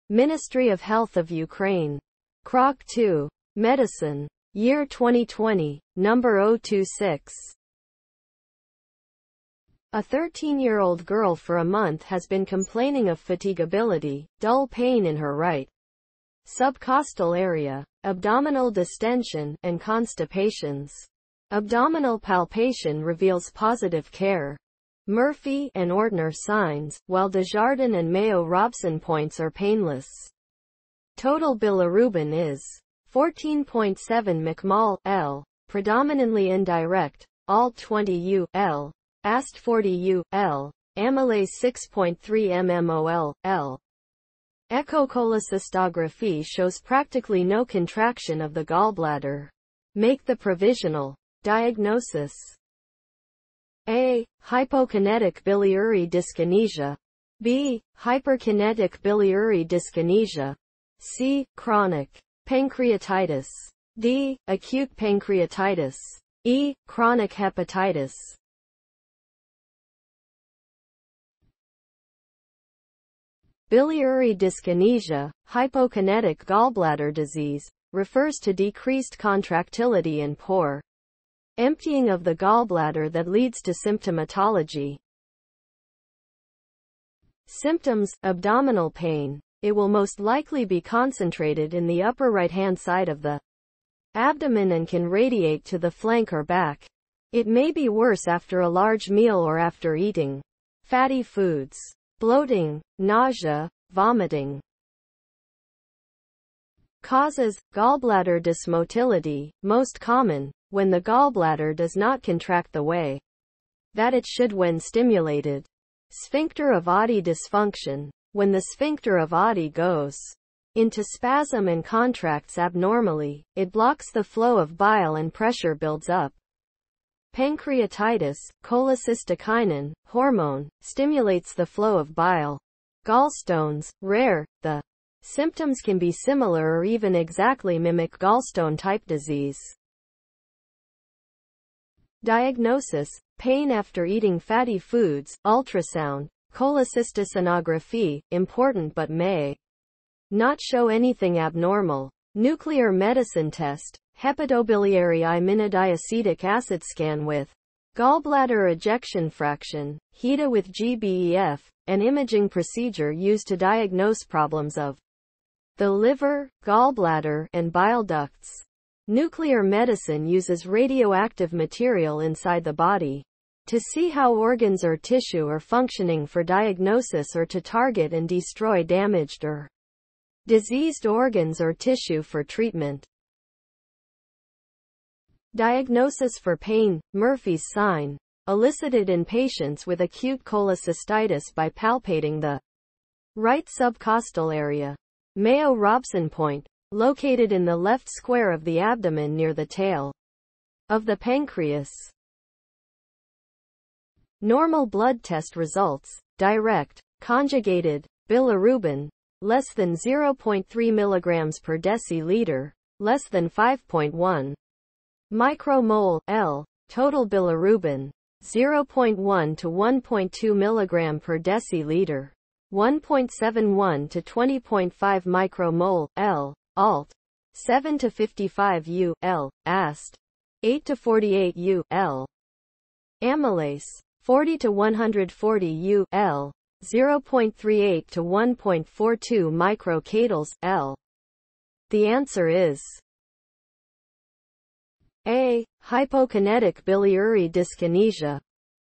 [ Krok 2 Medicine ] Year: 2020 - 026 (Ministry of Public Health of Ukraine)
A 13-year-old girl for a month has been complaining of fatigability, dull pain in her right subcostal area, abdominal distension, and constipations. Abdominal palpation reveals positive Kehr, Murphy, and Ortner signs, while Desjardins and Mayo-Robson points are painless. Total bilirubin is 14.7 mcmol/L, predominantly indirect, ALT- 20 U/L, AST- 40 U/L, amylase - 6.3 mmol/L. Echocholecystography shows practically no contraction of the gallbladder. Make the provisional diagnosis:

Acute pancreatitis
Chronic hepatitis
Chronic pancreatitis
Hyperkinetic biliary dyskinesia
Hypokinetic biliary dyskinesia